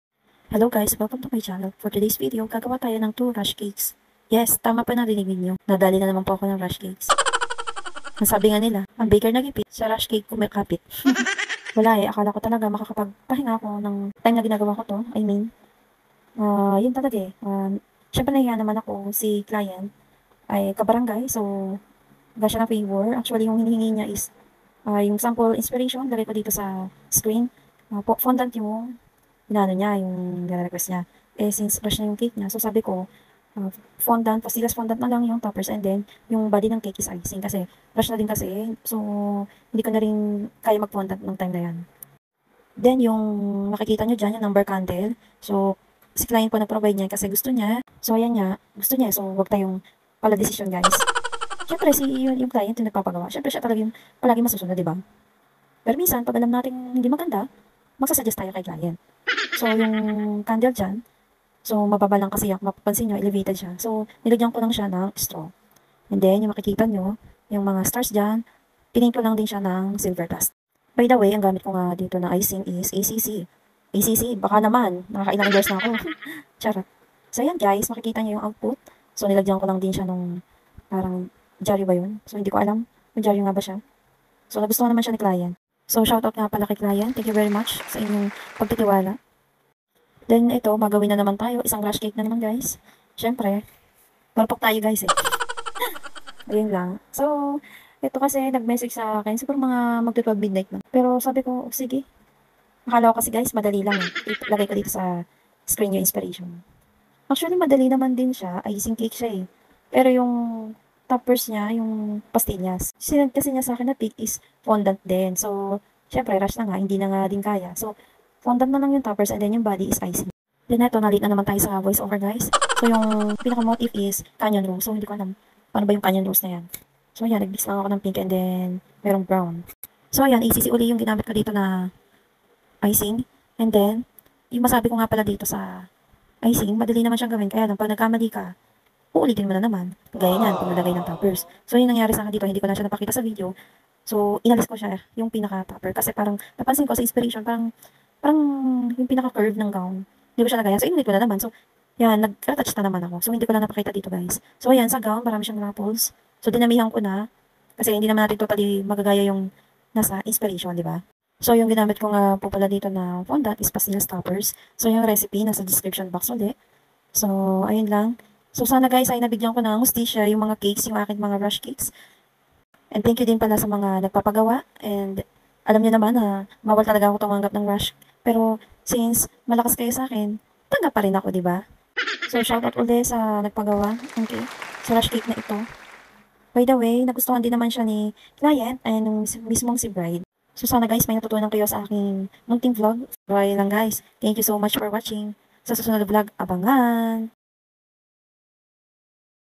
Weekend Rush Cakes (60th Birthday Cake and Bridal Shower Cake)
Hi guys! I'm going to share my weekend rush cake orders.

Tips: You can make toppers 3-4 days ahead before the pick-up date of the cake. Use room temp condensed milk and corn syrup. Store the toppers in air tight container at room temp.